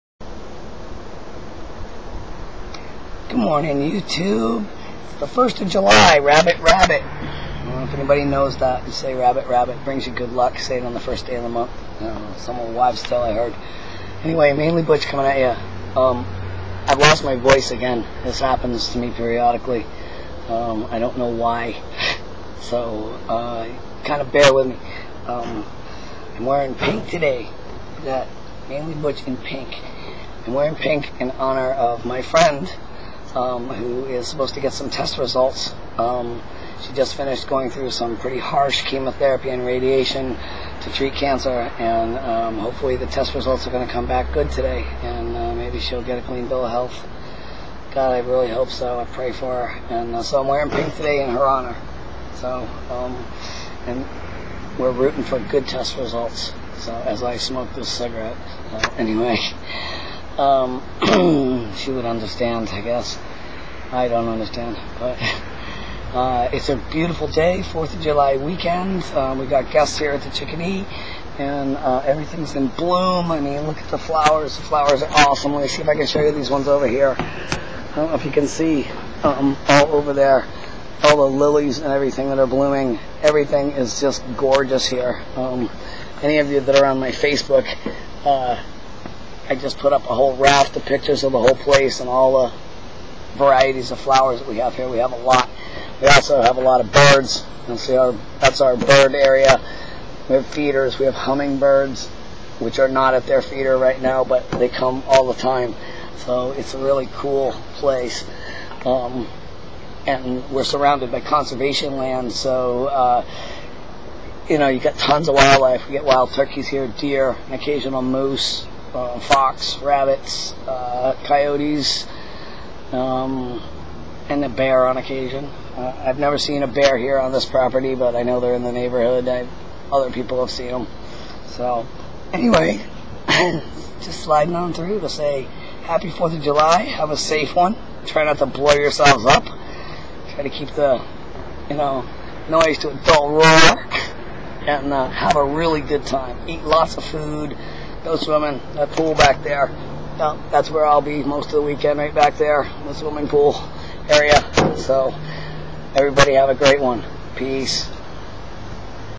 MainelyButch Slidin on Thru....
Quick hello, scene from the Chickadee B&B where I live, talkin about 4th of July and my friend who is supposed to get test results for her cancer treatment today.